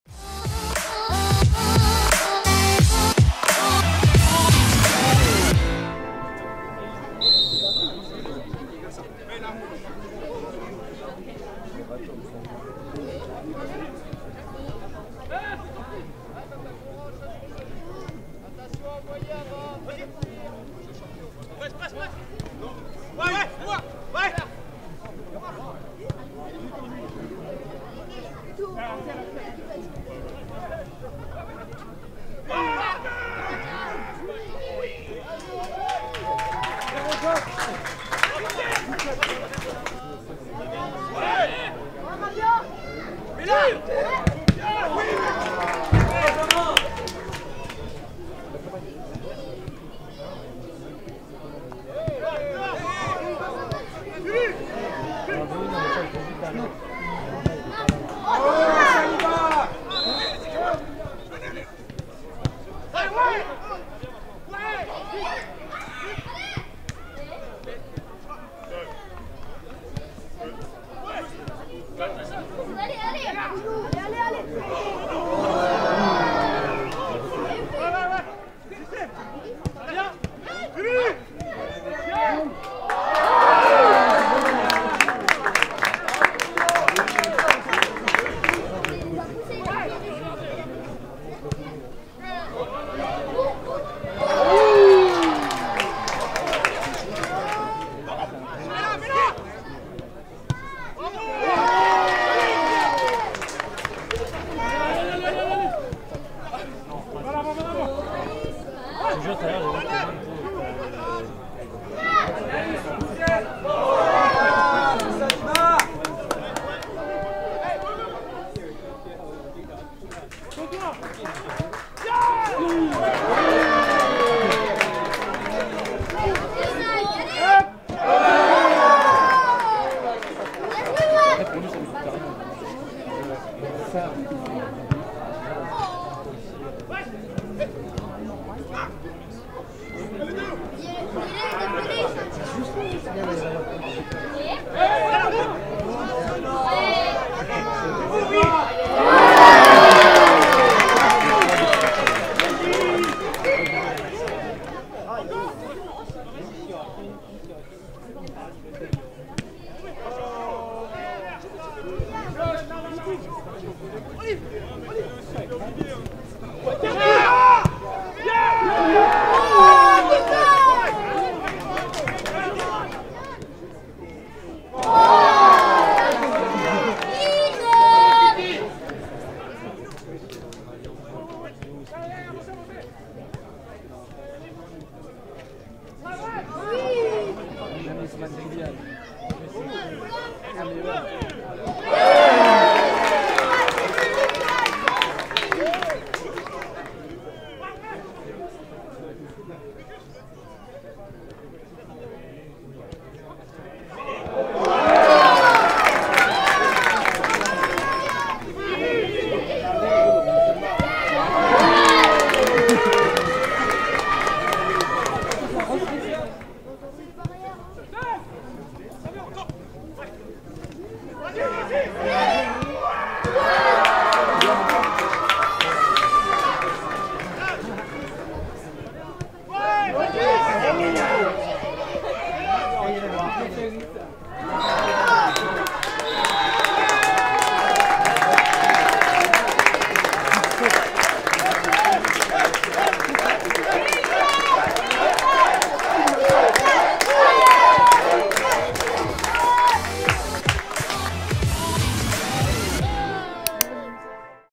Buts et actions, Equipe de France I FFF 2023
Les "Jaunes" de Kylian Mbappé ont remporté l'opposition sur terrain réduit mardi à Clairefontaine. Découvrez les actions et les buts.